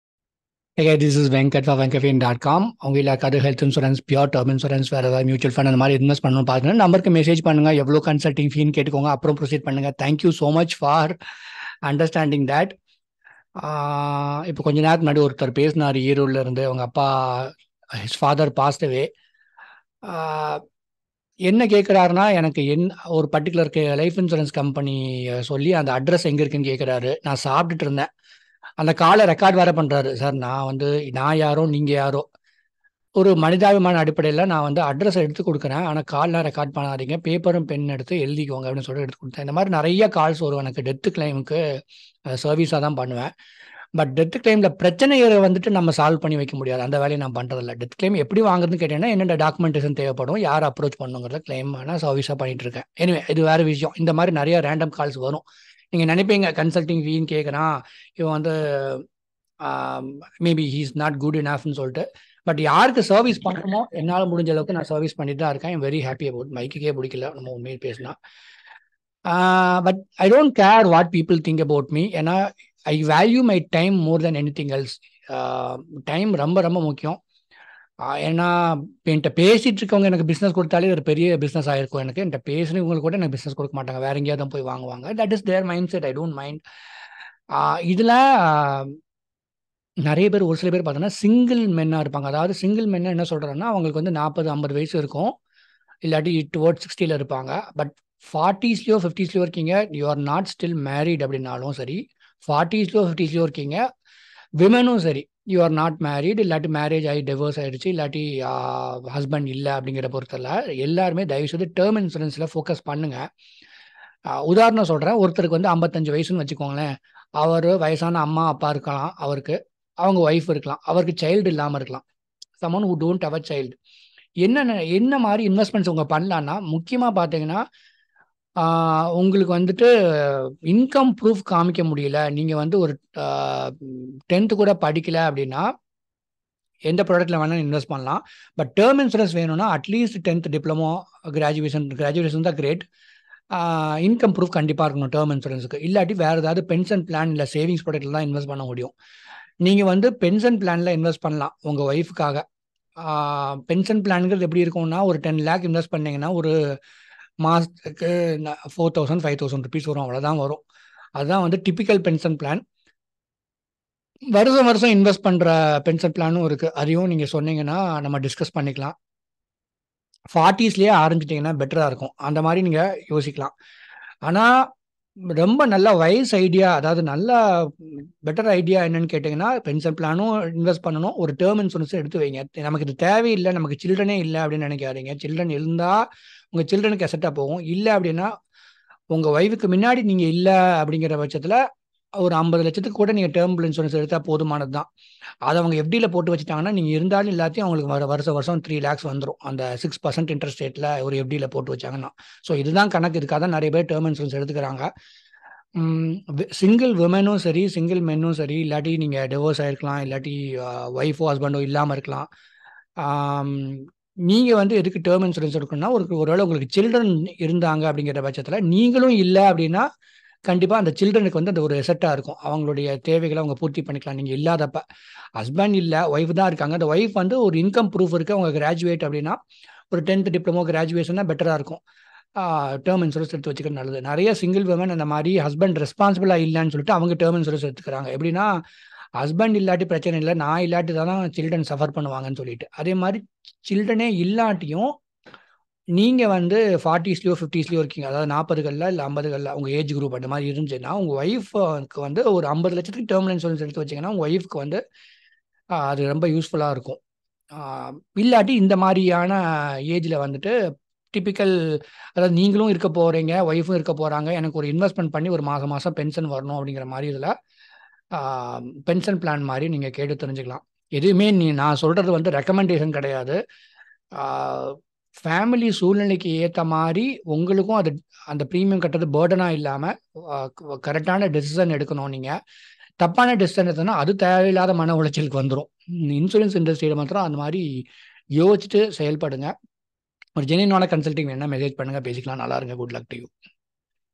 Term Insurance for Single Men & Single Women to protect your family | Tamil
What's benefit of having exclusive, personal health insurance?